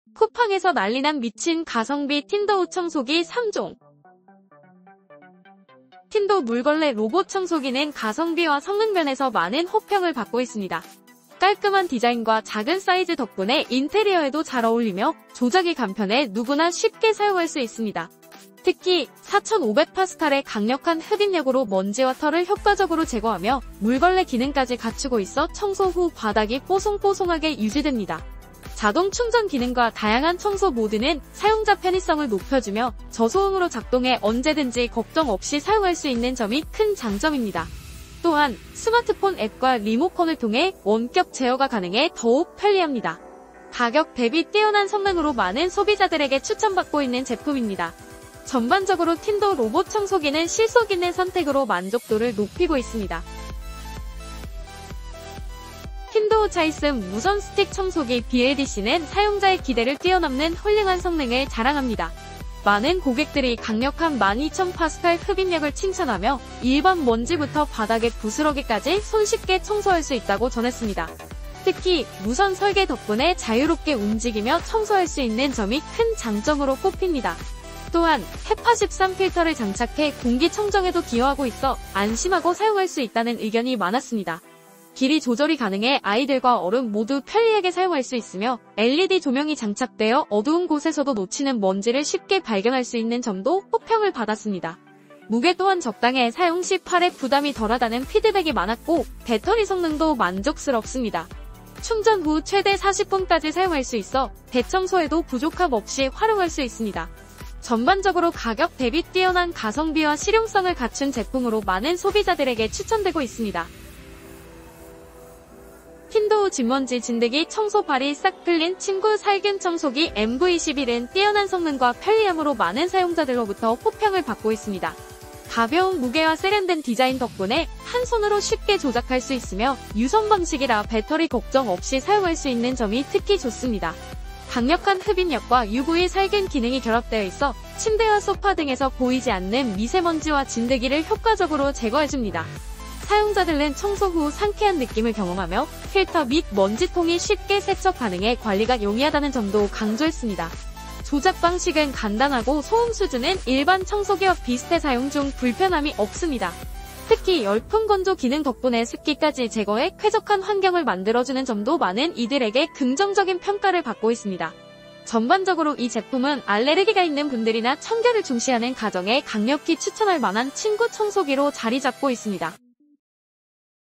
쿠팡에서 입소문난 미친 가성비! 틴도우 청소기 3종 추천 이건 안사면 손해
00:00 쿠팡에서 난리난 미친 가성비 틴도우청소기 3종

00:06 [2024년형]Teendow 물걸레 로봇청소기+물걸레 패드*4+필터*4+사이드 브러시*8+1년보증

00:55 [2024년형] 틴도우 차이슨 무선 스틱 청소기 BLDC 강력한 흡입력 Hepa13 필터

01:55 틴도우 집먼지 진드기 청소 바리 싹클린 침구 살균청소기 UV 유선 침구 청소기

안녕하세요! 오늘은 쿠팡에서 입소문난 미친 가성비 **Teendow 틴도우 청소기 3종**을 소개합니다.
가격 부담 없이 최고의 성능을 자랑하는 틴도우 청소기로, 여러분의 청소 시간을 확 줄여보세요.

1. **[2024년형] Teendow 물걸레 로봇청소기**
   - 물걸레 패드 4개, 필터 4개, 사이드 브러시 8개 포함
   - 1년 보증 제공

2. **[2024년형] 틴도우 차이슨 무선 스틱 청소기**
   - 강력한 BLDC 모터와 Hepa13 필터
   - 강력한 흡입력으로 먼지 제거에 탁월

3. **틴도우 집먼지 진드기 청소 바리 싹클린 침구 살균청소기**
   - UV 살균 기능과 강력한 흡입력으로 침구 속 진드기 제거
   - 유선 디자인으로 지속적인 강력한 성능 제공

이 영상에서는 각 제품의 특징과 장점을 비교해보며, 여러분의 가정에 가장 적합한 청소기를 추천해드립니다. Teendow 틴도우 청소기로 집안 청소를 쉽고 빠르게 해결해보세요!

👉 본 영상에는 채널의 주관적인 생각이 포함되어 있습니다.
👉 제품에 대하여 채널은 책임을 지지 않으며, 제품 관련은 쿠팡 고객센터로 연락 바랍니다.